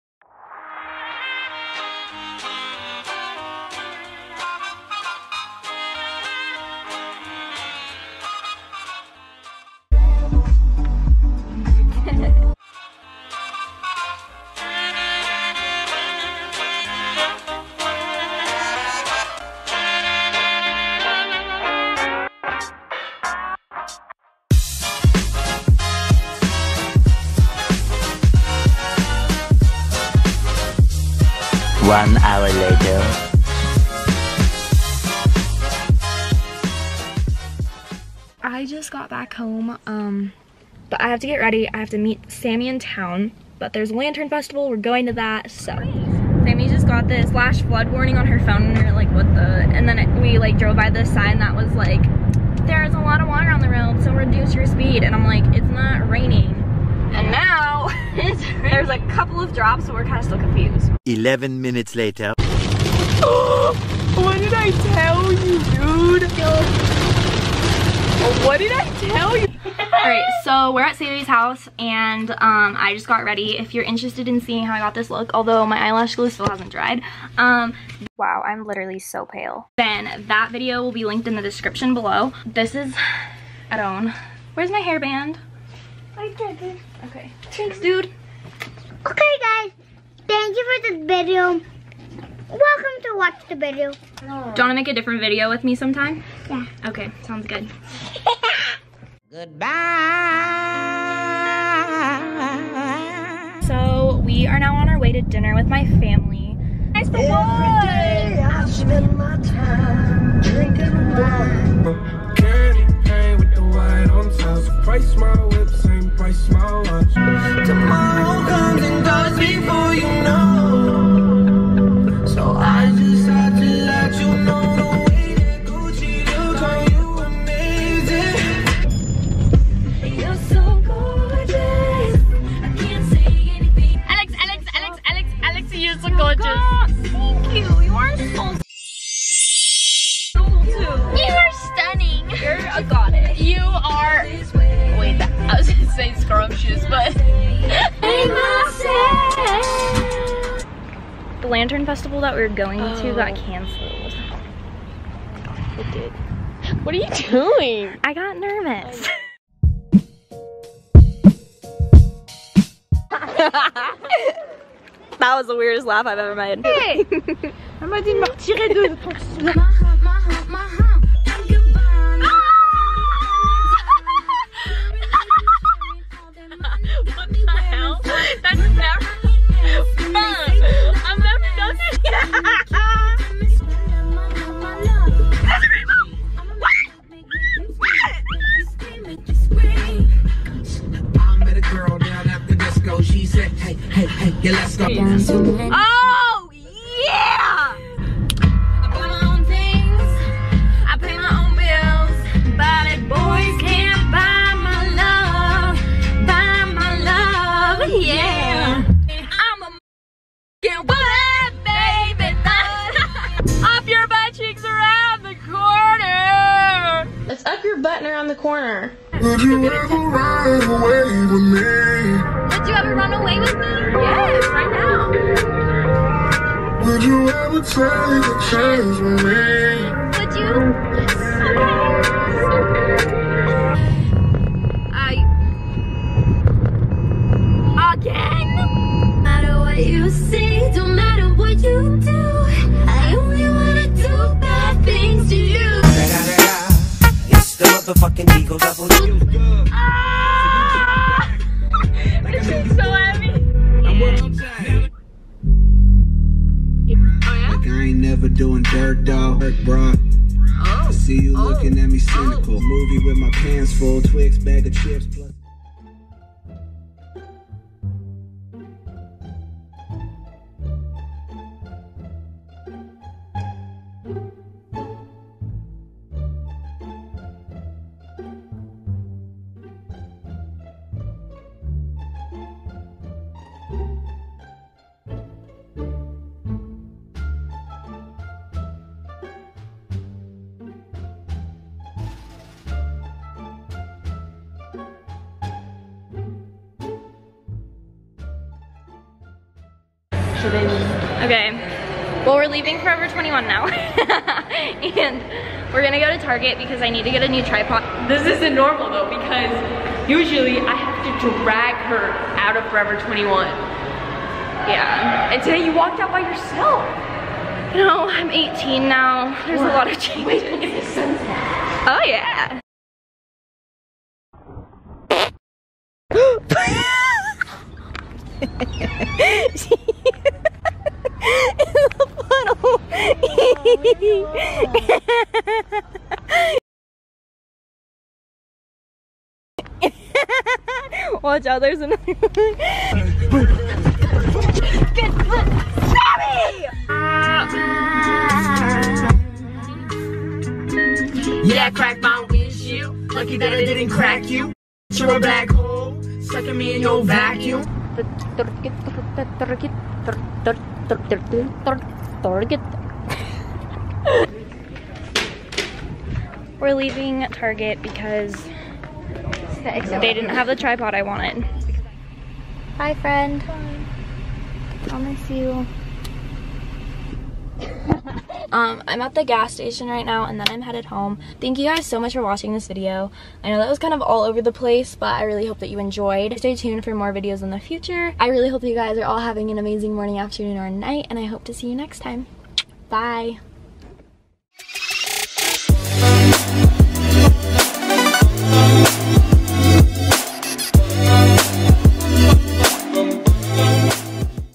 Day in my life// I ripped the dress!
I hope you enjoy this day in my life vlog!

If you haven't already seen it, check out my get ready with me!